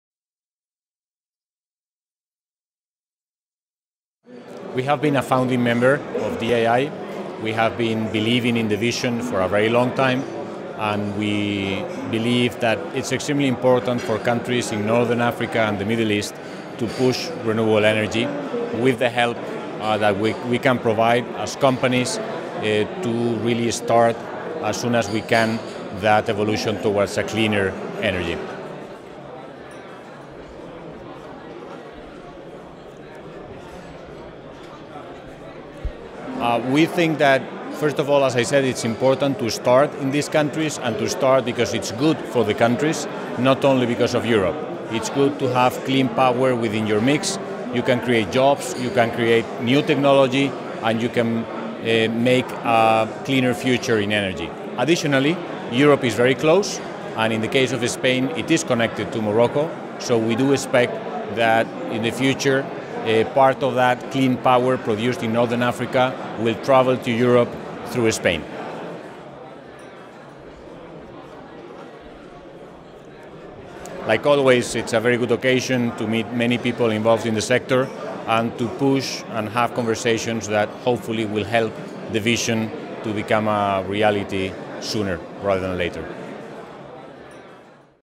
Dii Conference: Santiago Seage, CEO, Abengoa Solar (English)
Why did Abengoa Solar decide to get involved in Dii? What are the next steps that need to be taken for the realization of the Desertec vision? Santiago Seage, CEO of Abengoa Solar gives his opinion on these questions.